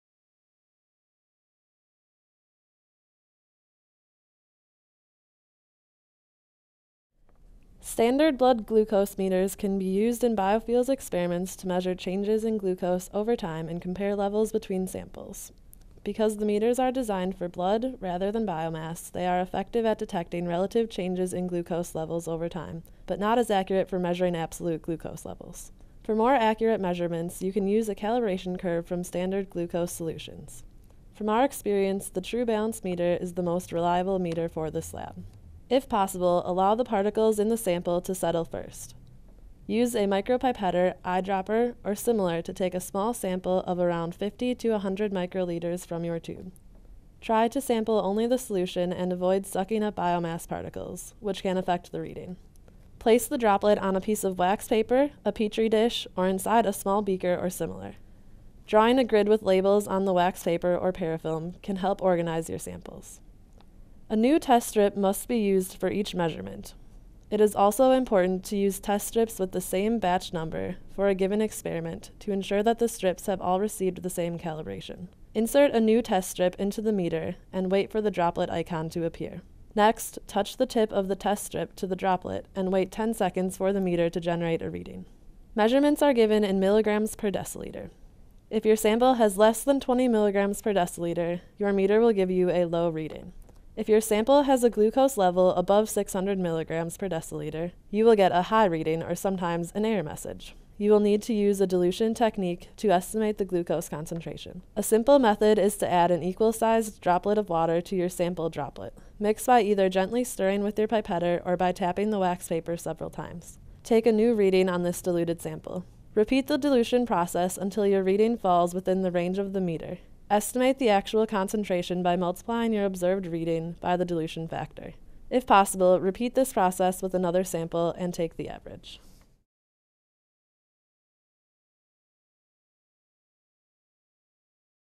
Blood Glucose Meters for Classroom Use: Tips & Tricks
Some of GLBRC's lab experiments require students to take glucose measurements to observe chemical processes like fermentation. In this video, a simple and inexpensive method is described for measuring glucose levels with standard blood glucose meters.